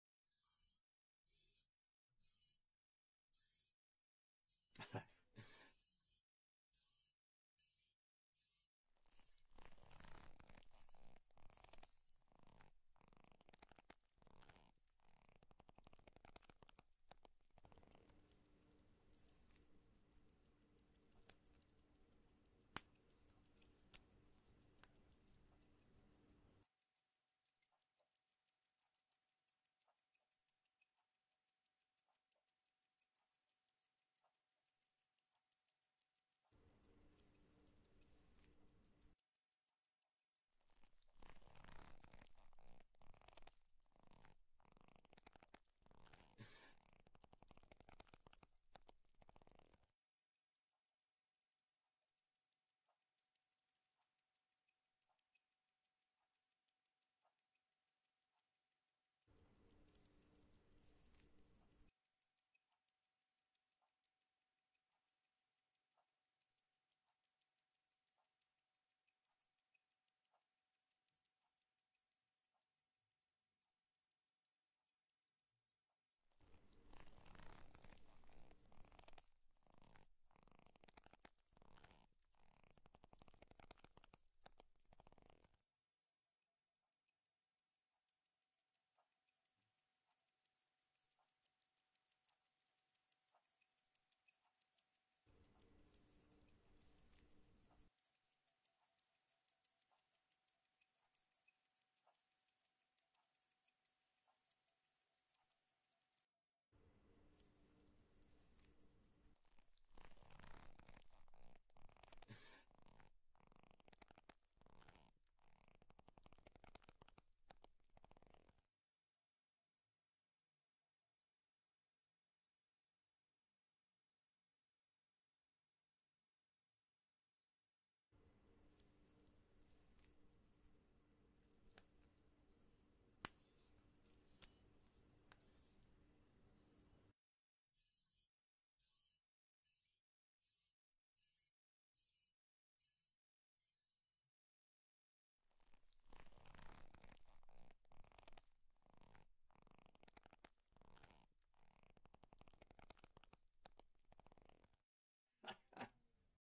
Watch Sounds | ASMR
Hear the faint tick tock of an automatic, leather strap, clasp, and old man laughing.